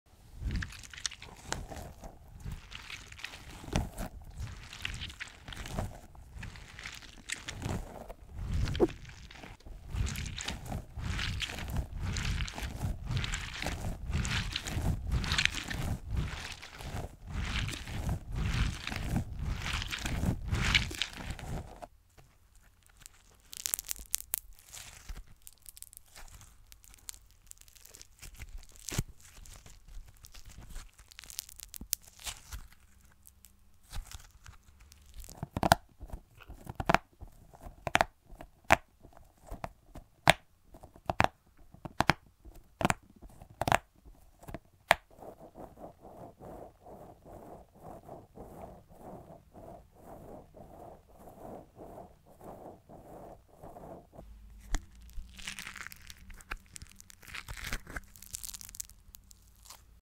ASMR by Jasmine Evanna
I went shopping at WHSmith this morning and got some fidget toys, and then realised that they would be really good for an ASMR video, so here's my first ever ASMR video!